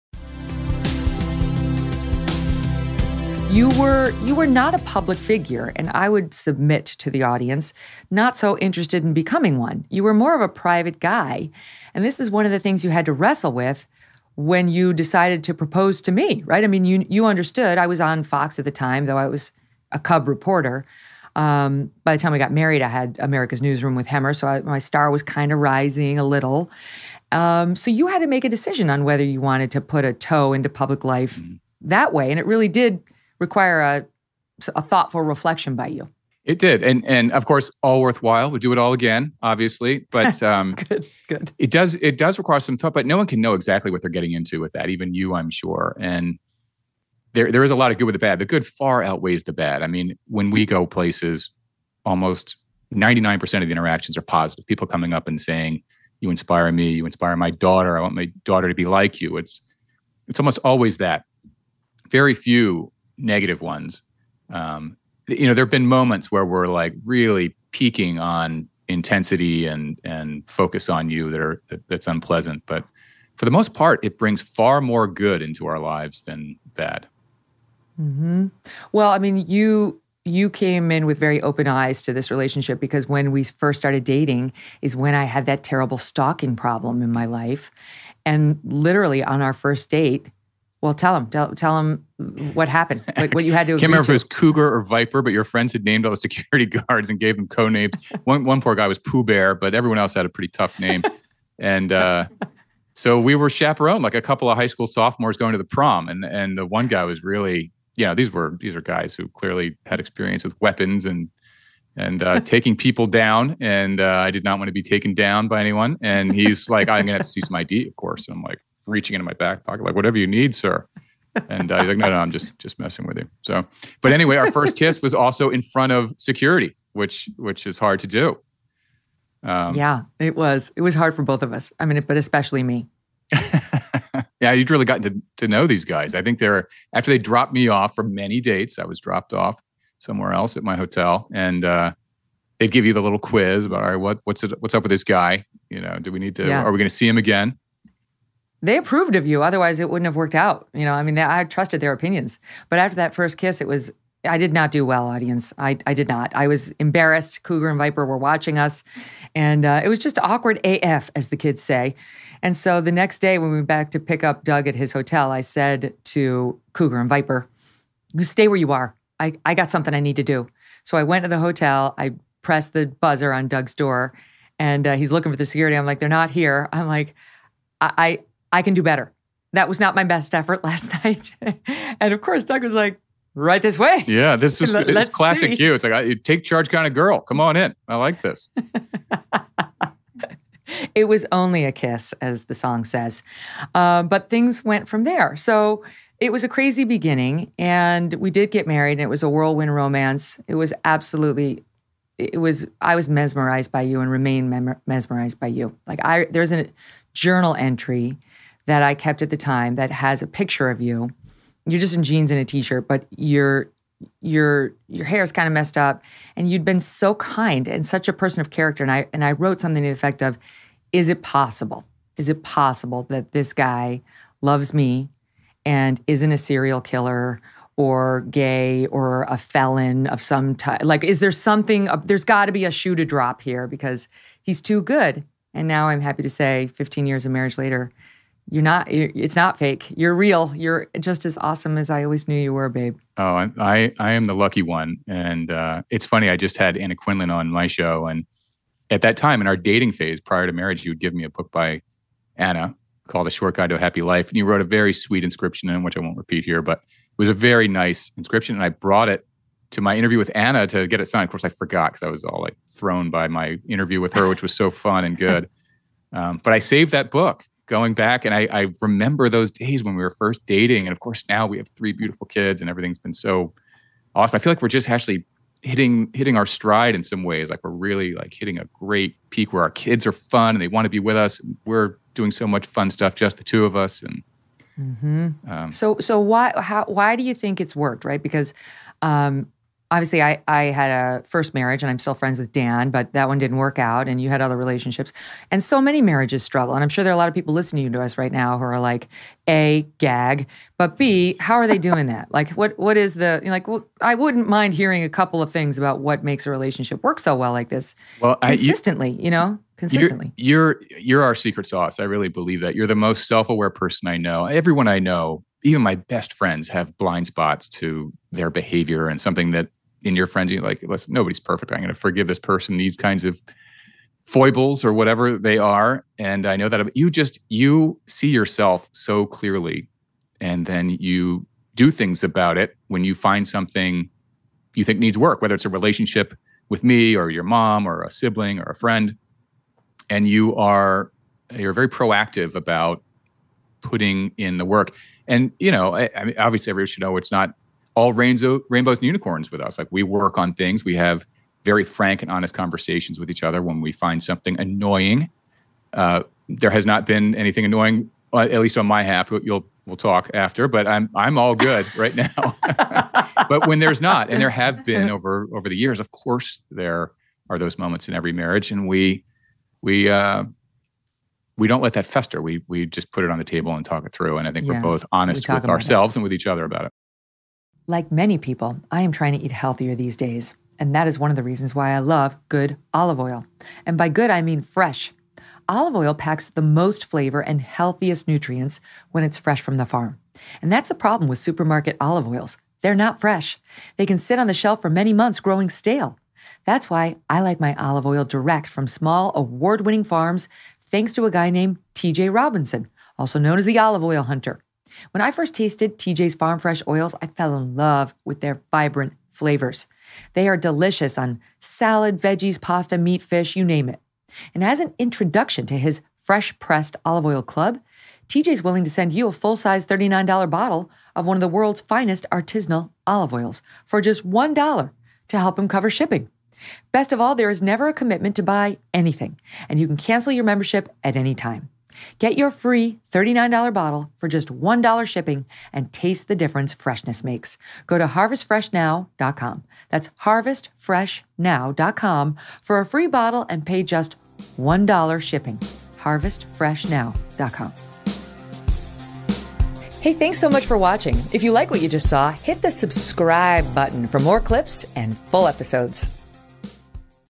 Megyn Kelly Talks to Her Husband Doug Brunt About What it Was Like to Date and Marry Her
Megyn Kelly is joined by her husband Doug Brunt, author and host of "Dedicated," to detail the origins of their relationship, what it was like to date and ultimately marry Megyn, Megyn’s initial hesitations that Doug was too good to be true, and more.